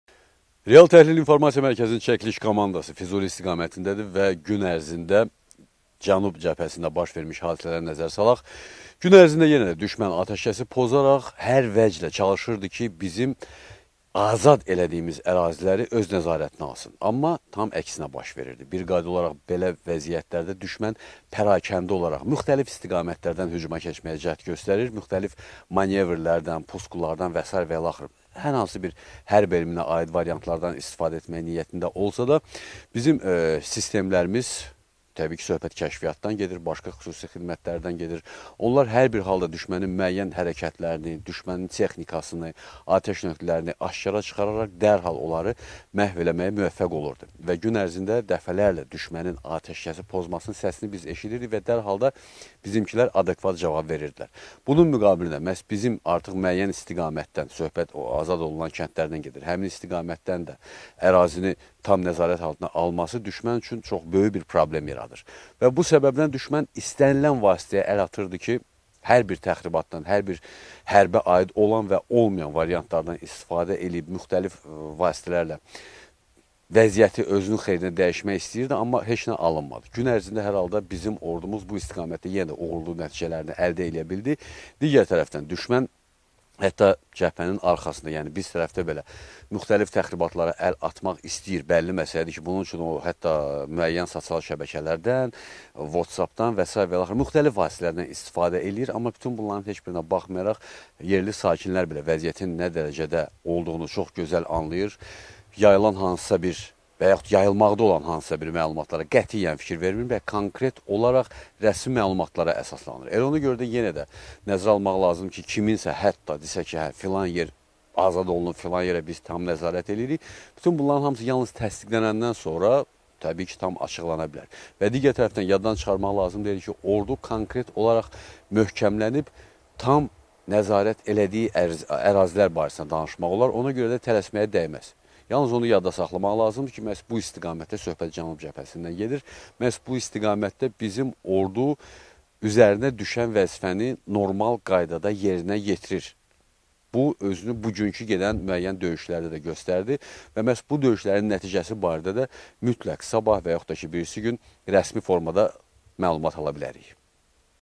Düşmən təxribatlarının qarşısı alınır
Cənub cəbhəsində düşmənin hər vasitəyə əl atması iflasa uğrayır